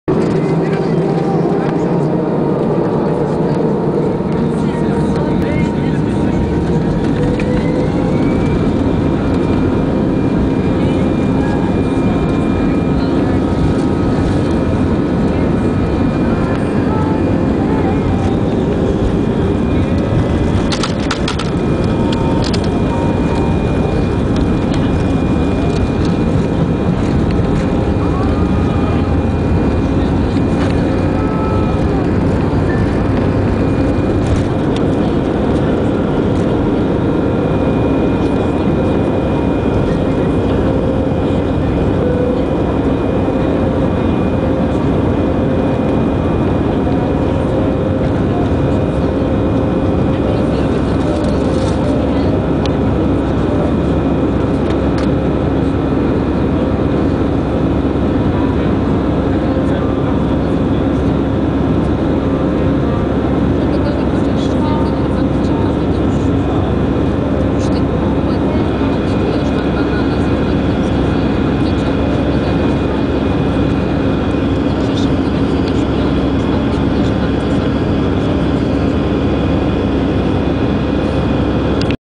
Finnair A319 takeoff from Warsaw
Night takeoff from Warsaw Okecie airport to Helsinki Vantaa. Finnair Airbus A319 OH-LVD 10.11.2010